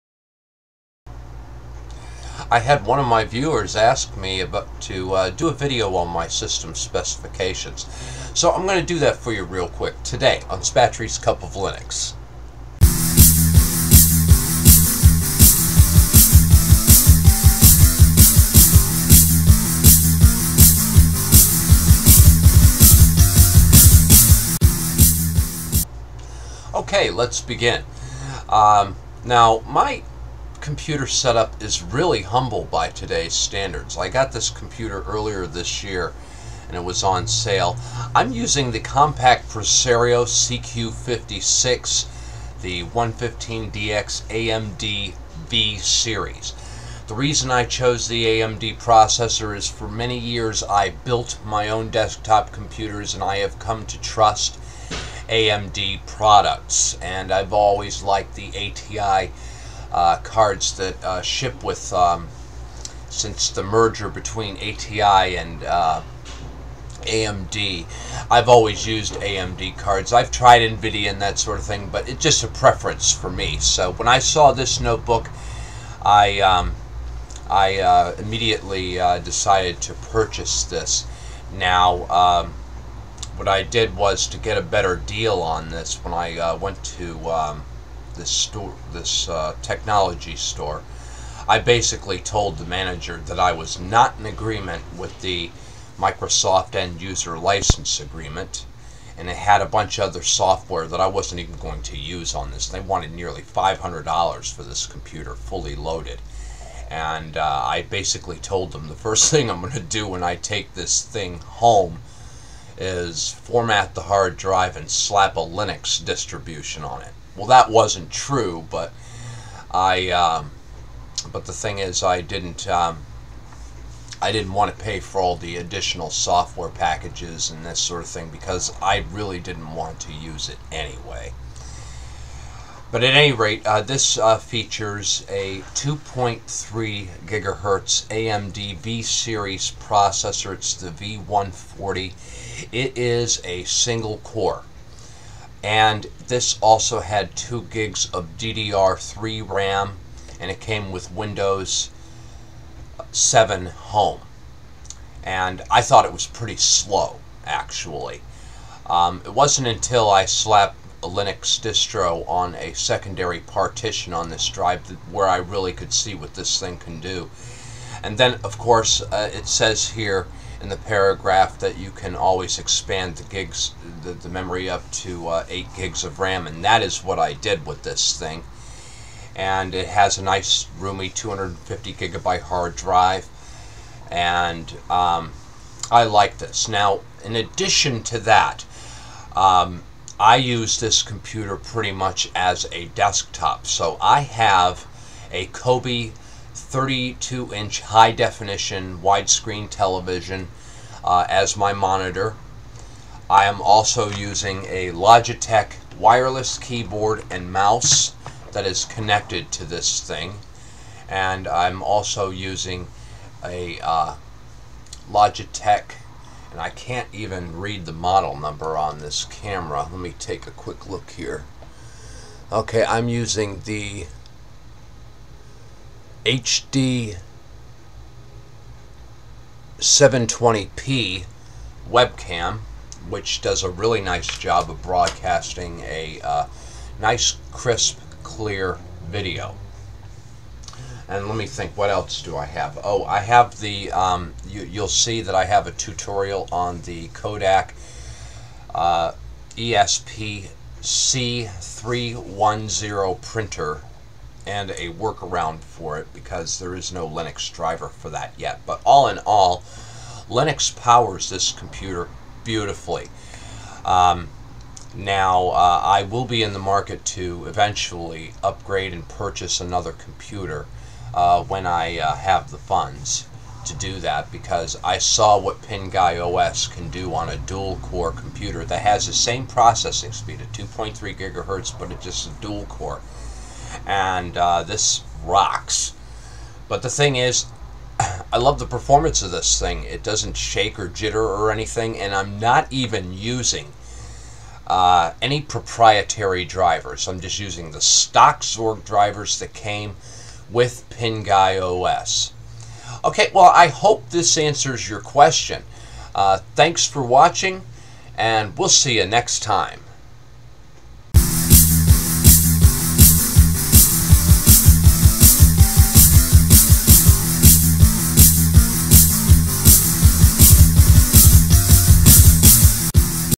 Spatry's Computer SPECS
In this episode, I will detail my system specifications for you.